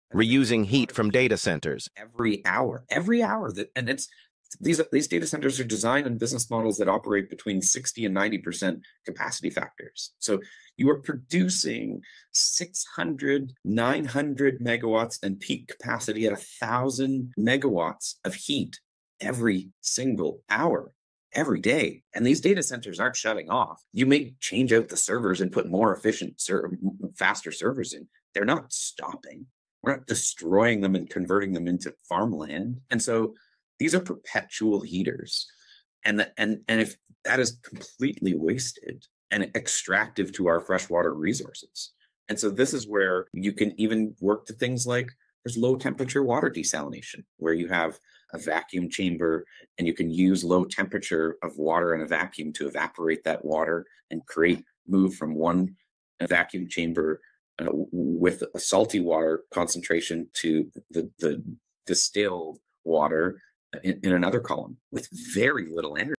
🔥Data Centers Waste Gigawatts of Heat?
Data centers generate gigawatts of wasted heat every hour, running at 60-90% capacity. Scot Bryson, CEO of Orbital Farm, highlights the unsustainable energy consumption and water waste.